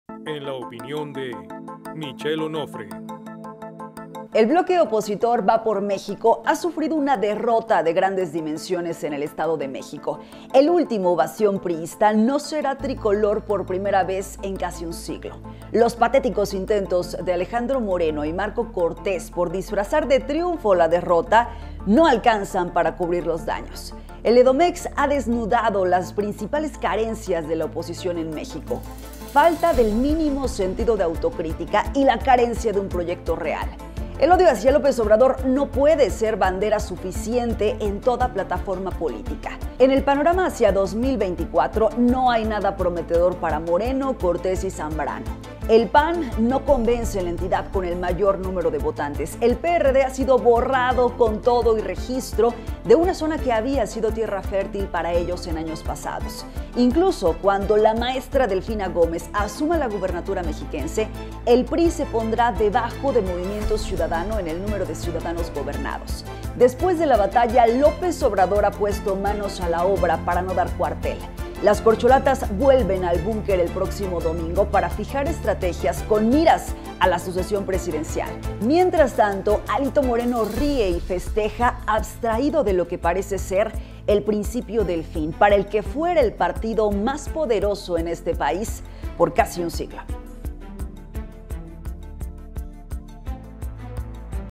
Edomex, el principio del fin
La derrota del PRI en el Estado de México destapó algunas de las carencias más significativas (y nocivas) de la oposición en el país. Hablemos al respecto.

ESTA ES LA OPINIÓN DE... Michelle Onofre